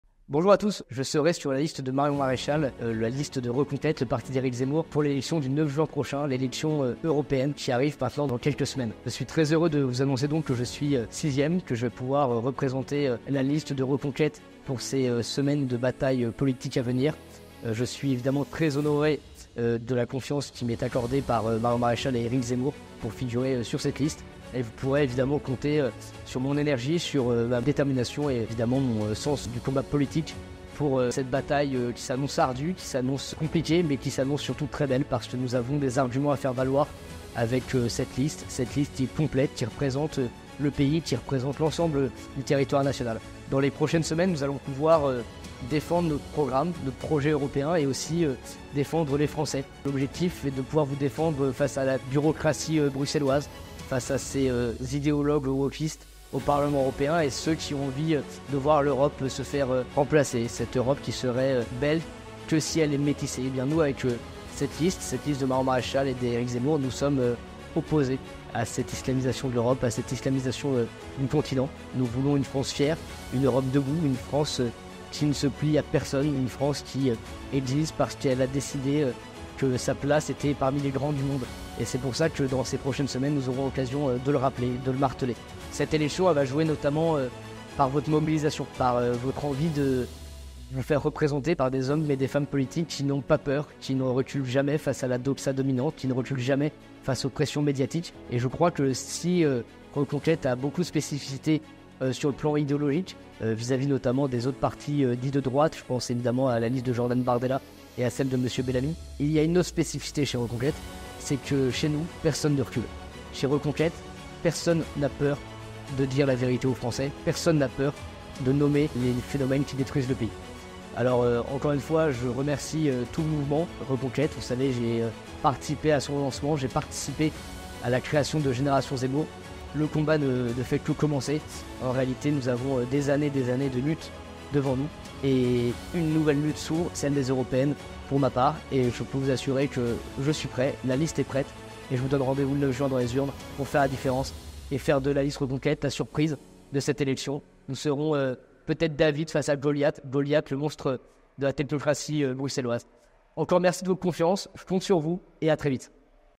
Stanislas Rigault : Je rejoins la liste de Reconquête aux élections européennes !
Stanislas Rigault, président de Génération Zemmour et membre du bureau exécutif de Reconquête, vous annonce sa candidature aux élections européennes sur la liste de Reconquête, conduite par Marion Maréchal et soutenue par Éric Zemmour.

4️⃣ Recevez toutes les informations en avant-première

5️⃣ Suivez Éric Zemmour : zemmour2022.fr